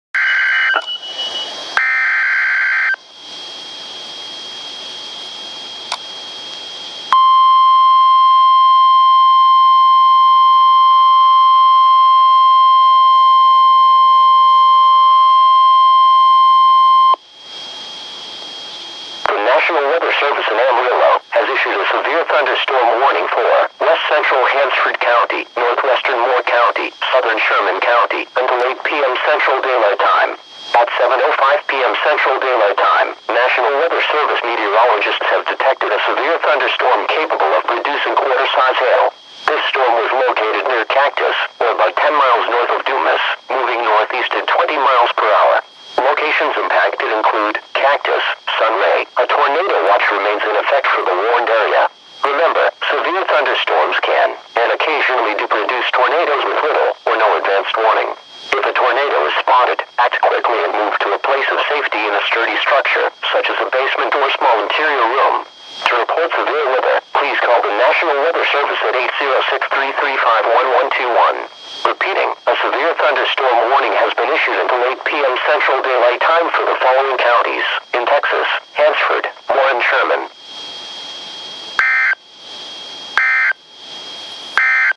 Severe Thunderstorm Warning #2 in Texas - 04/11/12
WXK38 Amarillo, TX.

Text:
NATIONAL WEATHER SERVICE AMARILLO TX
705 PM CDT WED APR 11 2012

THE NATIONAL WEATHER SERVICE IN AMARILLO HAS ISSUED A

  WEST CENTRAL HANSFORD COUNTY IN THE PANHANDLE OF TEXAS
  NORTHWESTERN MOORE COUNTY IN THE PANHANDLE OF TEXAS
  SOUTHERN SHERMAN COUNTY IN THE PANHANDLE OF TEXAS

* UNTIL 800 PM CDT

* AT 705 PM CDT...NATIONAL WEATHER SERVICE METEOROLOGISTS HAVE
  DETECTED A SEVERE THUNDERSTORM CAPABLE OF PRODUCING QUARTER SIZE
  HAIL.  THIS STORM WAS LOCATED NEAR CACTUS...OR ABOUT 10 MILES NORTH
  OF DUMAS...MOVING NORTHEAST AT 20 MPH.

  CACTUS...
  SUNRAY...

PRECAUTIONARY/PREPAREDNESS ACTIONS...

A TORNADO WATCH REMAINS IN EFFECT FOR THE WARNED AREA. REMEMBER...
SEVERE THUNDERSTORMS CAN AND OCCASIONALLY DO PRODUCE TORNADOES WITH
LITTLE OR NO ADVANCED WARNING. IF A TORNADO IS SPOTTED...ACT QUICKLY
AND MOVE TO A PLACE OF SAFETY IN A STURDY STRUCTURE...SUCH AS A
BASEMENT OR SMALL INTERIOR ROOM.

TO REPORT SEVERE WEATHER...PLEASE CALL THE NATIONAL WEATHER SERVICE
AT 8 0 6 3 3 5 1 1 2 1.